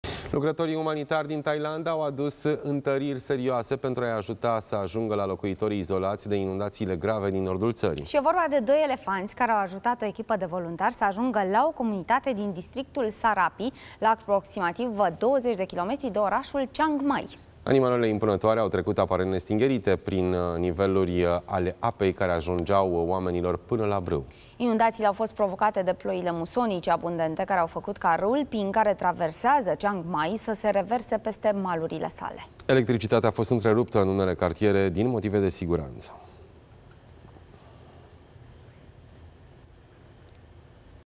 Doi elefanți au ajutat voluntarii la inundațiile din Thailanda
Elefanții contribuie la eforturile de ajutorare în inundatiile din Thailanda